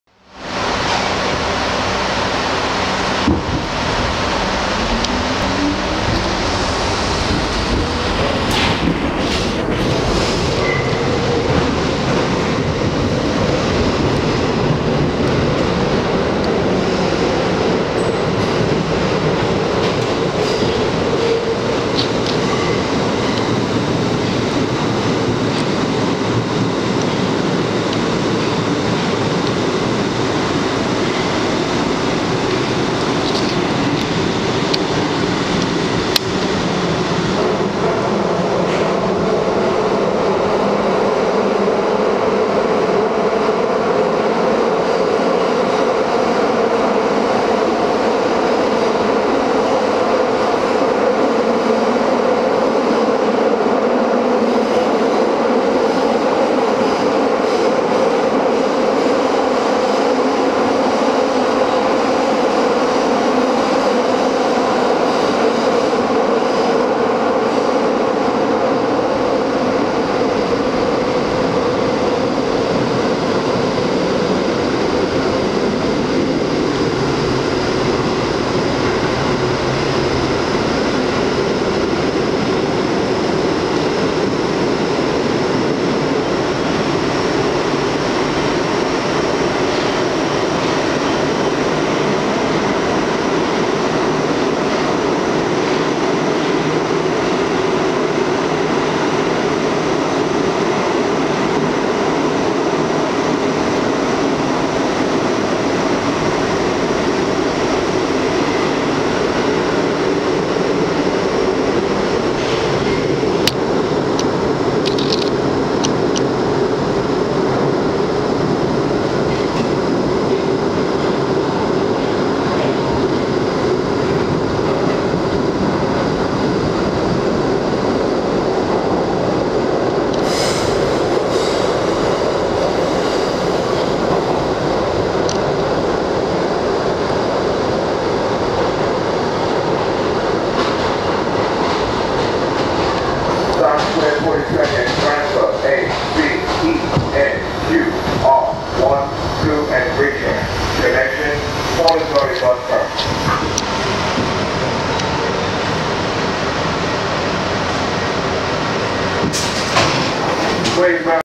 Alone On The R62A (7) Train From 34th Street To 42nd Street-Times Square
New York City Subway video of riding the empty R62A 7 Train from 34th Street-Hudson Yards to 42nd Street-Times Square. Video was recorded on 7/1/2016 with the Nikon D610.